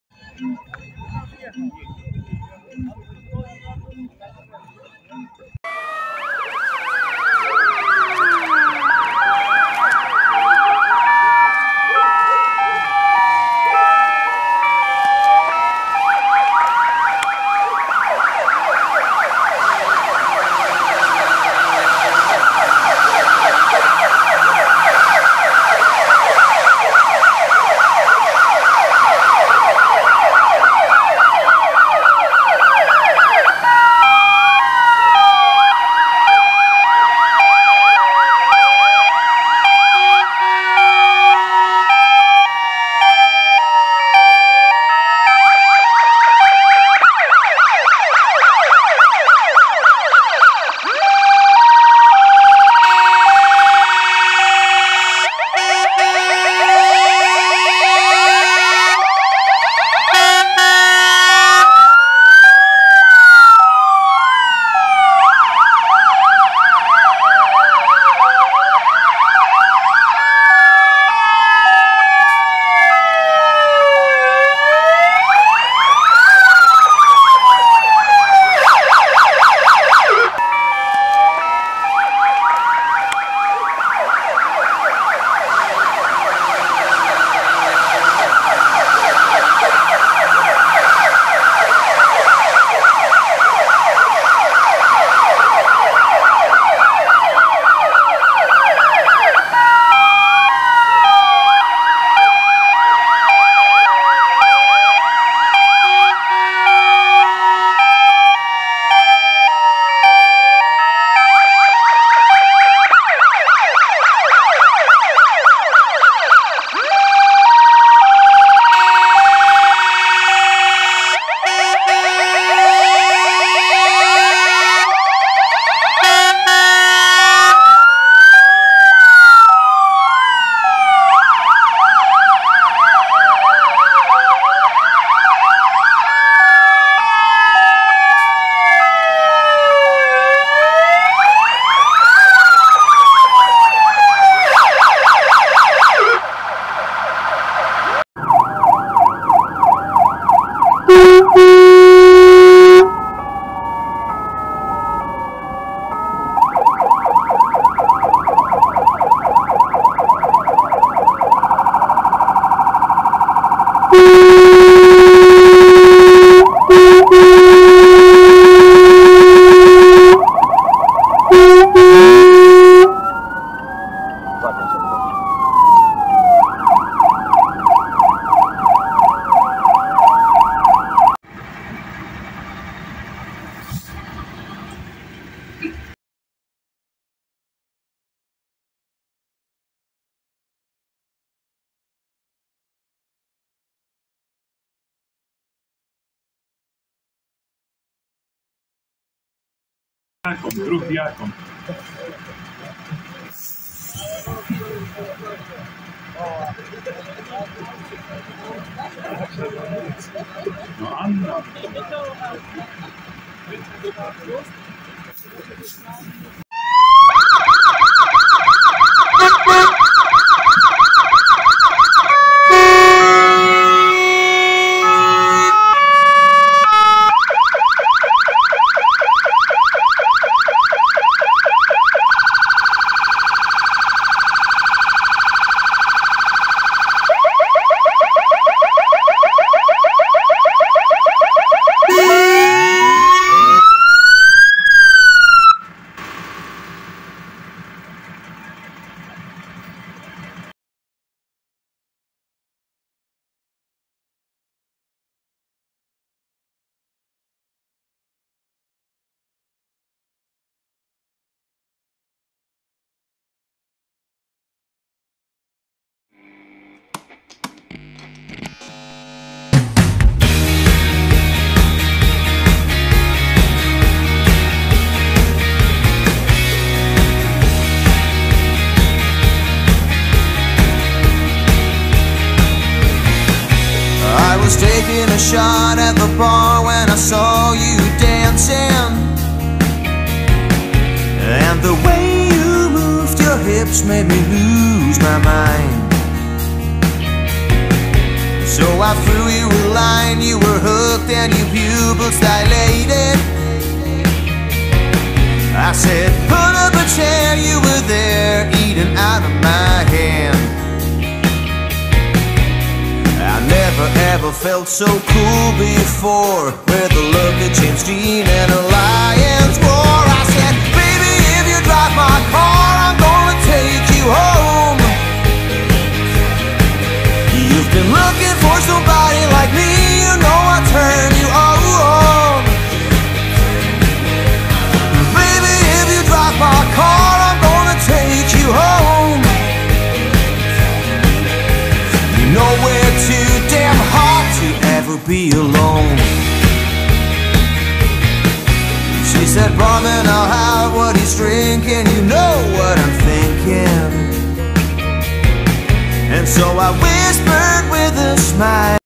Huczne powitanie nowego wozu w OSP KSRG Mołodycz! 439[R]34 GBARt Renault Range D16/ Moto-Truck.
W niedzielę 5 lutego br. miał miejsce historyczny moment dla strażaków z OSP KSRG Mołodycz. Strażacy hucznie powitali swój nowy historyczny samochód. Jest nim Renault D16 w zabudowie firmy Moto-Truck. Dziękuję bardzo strażakom za współpracę oraz za zaproszenie na te wydarzenie. Niedługo myślę uda się ogarnąć fajną sesję w terenie z ich nowym nabytkiem.
Druhom gratuluję pozyskania nowego wozu oby godnie służył.

Zapraszam również na stronę na  Facebook gdzie niedługo ukażą się zdjęcia z tego wydarzenia jak i wozu.